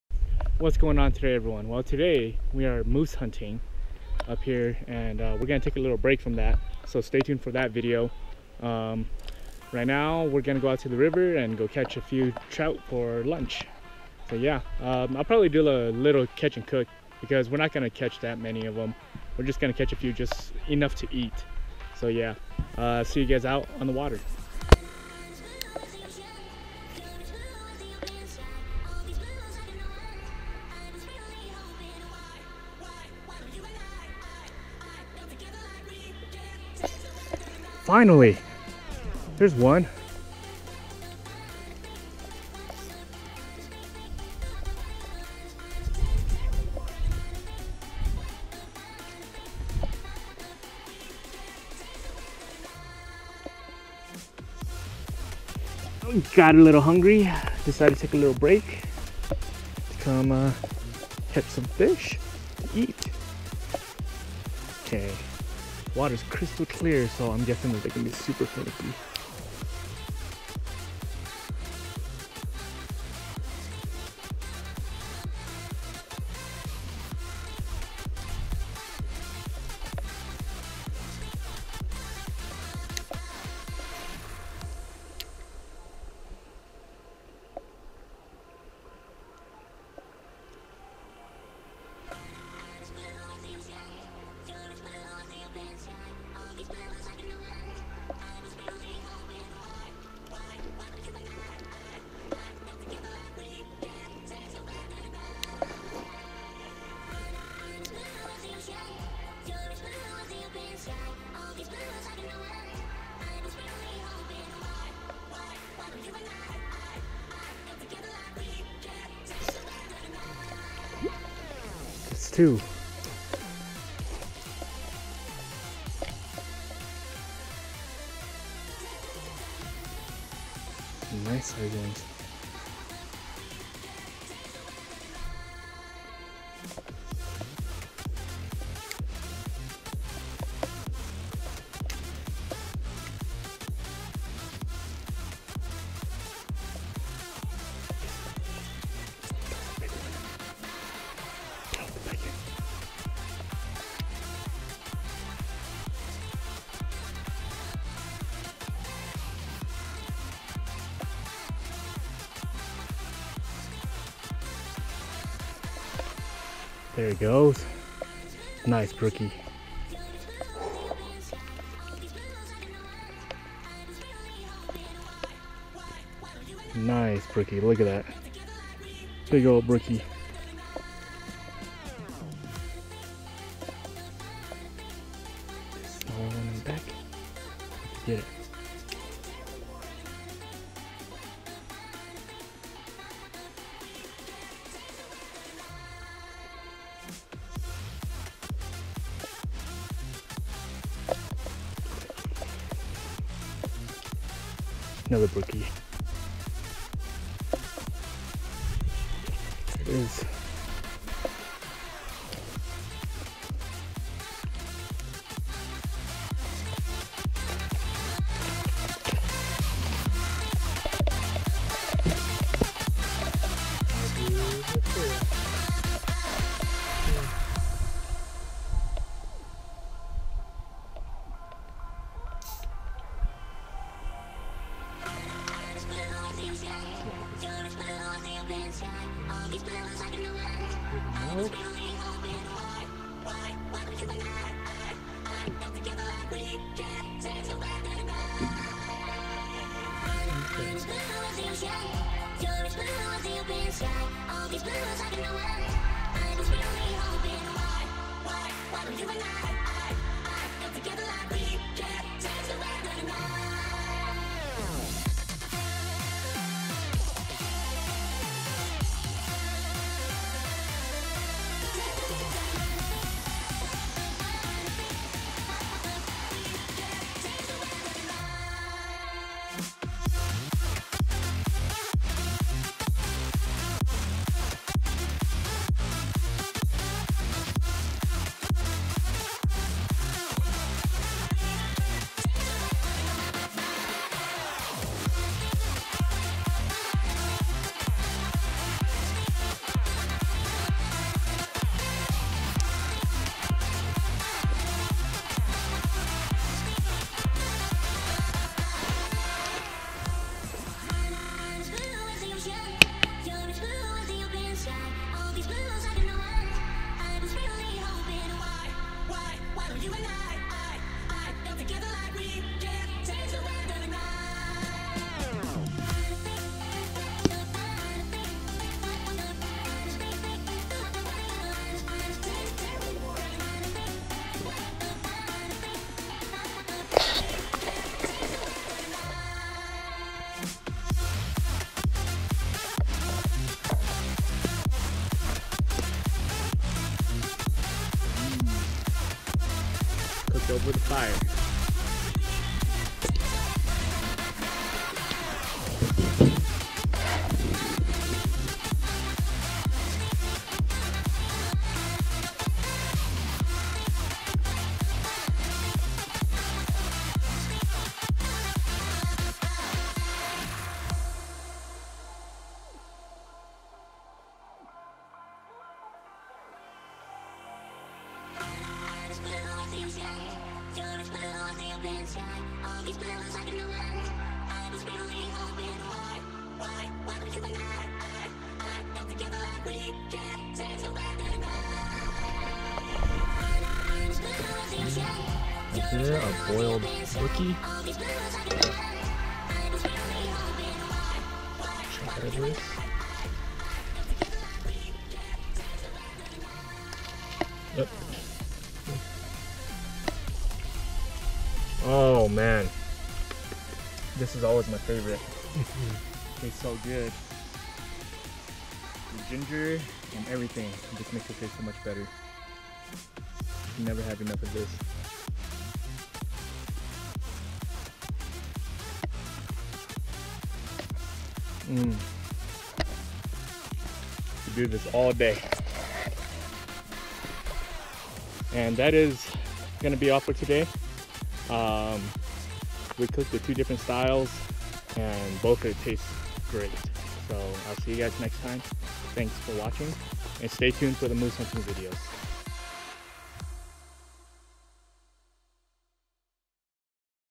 CATCH AND COOK brook trout stream fishing
took a little break from hunting moose to catch myself some lunch =)

need to get a hold of me ? or just want to stop by and say hi ?